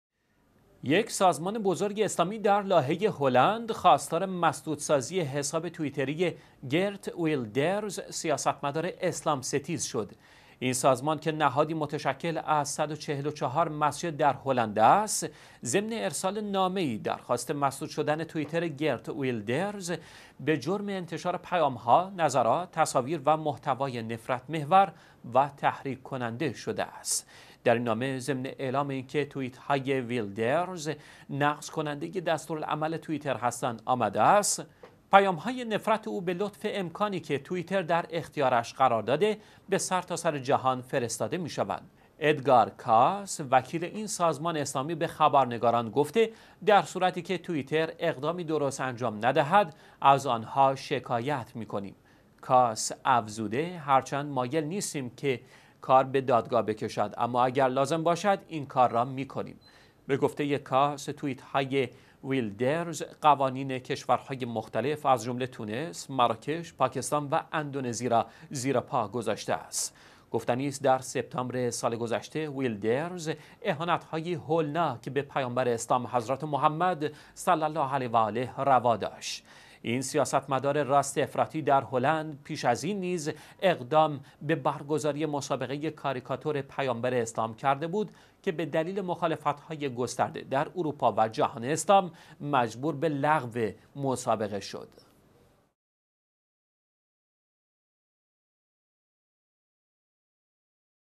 درخواست مسلمانان هلند برای مسدود شدن حساب توییتری یک اسلام ستیز
یک سازمان بزرگ اسلامی  در «لاهه» هلند، خواستار مسدودسازی حساب توییتری «گرت ویلدرز» سیاستمدار اسلام ستیز شد.
 این سازمان که نهادی متشکل از 144 مسجد در هلند است، ضمن ارسال نامه ای درخواست مسدود شدن توییتر گرت ویلدرز به جرم انتشار پیام ها، نظرات، تصاویر و محتوای نفرت محور و تحریک کننده شده است.
در این نامه، ضمن اعلام اینکه توییت های ویلدرز، نقض کننده دستورالعمل توییتر هستند، آمده است: پیام های نفرت او، به لطف امکانی که توییتر در اختیارش قرار داده، به سرتاسر جهان فرستاده می شوند.
«ادگار کاس» وکیل این سازمان اسلامی، به خبرنگاران گفته در صورتی که توییتر اقدامی درست انجام ندهد، از آن ها شکایت می کنیم.
کاس افزوده هرچند مایل نیستیم کار به دادگاه بکشد، اما اگر لازم باشد این کار را می کنیم.
به گفته کاس، توییت های ویلدرز، قوانین کشورهای مختلف از جمله تونس، مراکش، پاکستان و اندونزی را زیر پا گذاشته است.
گفتنی است در سپتامبر سال گذشته، ویلدرز اهانت هایی هولناک به پیامبر اسلام حضرت «محمد صلی الله علیه و آله»  روا داشت.
این سیاستمدار راست افراطی در هلند، پیش از این نیز اقدام به برگزاری مسابقه کاریکاتور پیامبر اسلام کرده بود که به دلیل مخالفت های گسترده در اروپا و جهان اسلام، مجبور به لغو مسابقه شد.

چهارشنبه - ۱۶ آبان ۱۳۹۷
الأربعاء - ٢٨ صفر ١٤٤٠